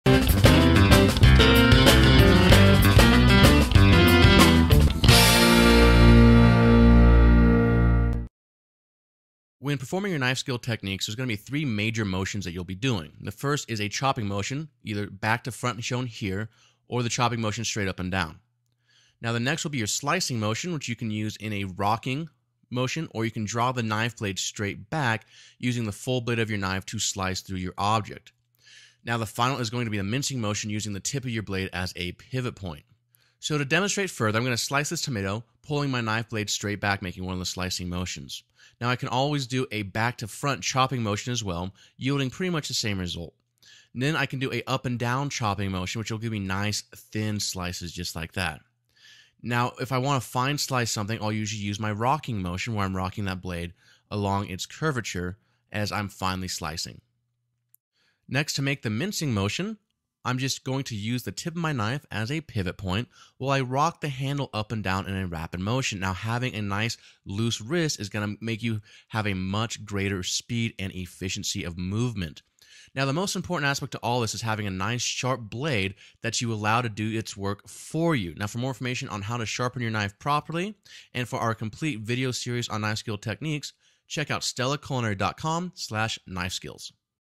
The Three Major Culinary Knife Cuts - Learn How to Slice, Dice & Mince Like a Professional Chef
About 95% of all knife skills techniques use one of three cuts. Master and understand these cuts, and you'll be well on your way to developing great, professional, knife skills.